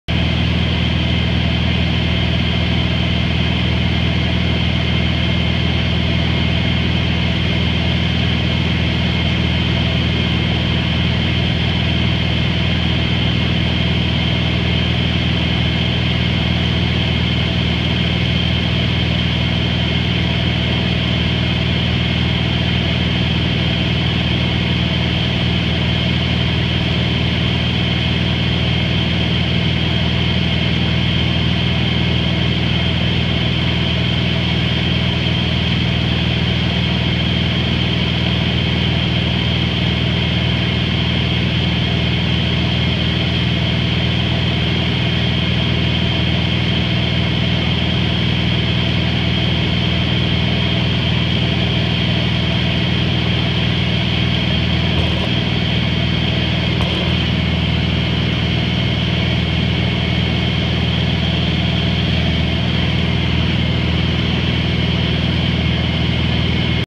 Monterey Bay Flyover (San Carlos, CA to Salinas, CA) AOPA Fly-in
quick flyover of Monterey Bay on the way from San Carlos to the AOPA Fly-in Salinas, CA